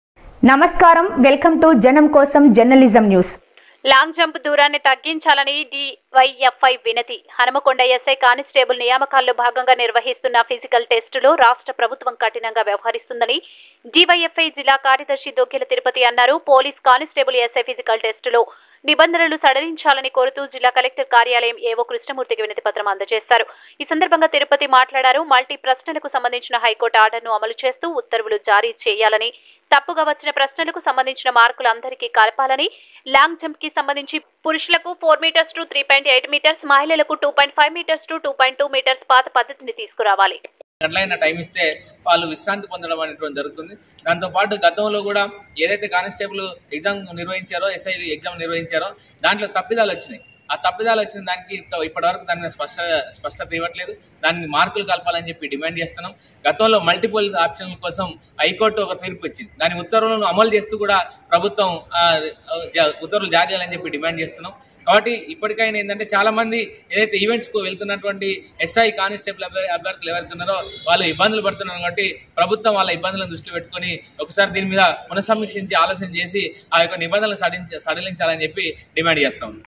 హనుమకొండ: ఎస్సై, కానిస్టేబుల్ నియామకాల్లో భాగంగా నిర్వహిస్తున్న ఫిజికల్ టెస్టులో రాష్ట్ర ప్రభుత్వం
*లాంగ్ జంప్ దూరాన్ని తగ్గించాలనీ డివైఎఫ్ఐ వినతి*
హనుమకొండ: ఎస్సై, కానిస్టేబుల్  నియామకాల్లో భాగంగా నిర్వహిస్తున్న ఫిజికల్ టెస్టులో రాష్ట్ర ప్రభుత్వం కఠినంగా వ్యవహరిస్తుందని డివైఎఫ్ఐ జిల్లా కార్యదర్శి దోగ్గెల తిరుపతి అన్నారు.
పోలీస్ కానిస్టేబుల్, ఎస్సై ఫిజికల్ టెస్టులో  నిబంధనలు  సడలించాలని కోరుతూ జిల్లా కలెక్టర్ కార్యాలయంలో ఏవో కృష్ణమూర్తికి వినతిపత్రం అందజేశారు.
ఈ సందర్భంగా తిరుపతి మాట్లాడారు మల్టీ ప్రశ్నలకు సంబంధించిన హైకోర్టు ఆర్డర్ను అమలు చేస్తూ ఉత్తర్వులు జారీ చేయాలని,తప్పుగా వచ్చిన ప్రశ్నలకు  సంబంధించిన మార్కులు అందరికీ కలపాలనీ,
*లాంగ్ జంప్ కి సంబంధించి పురుషులకు 4mts  to 3.8 mts , మహిళలకు 2.5mts to 2.2mts  పాత పద్ధతిని తీసుకురావాలి*.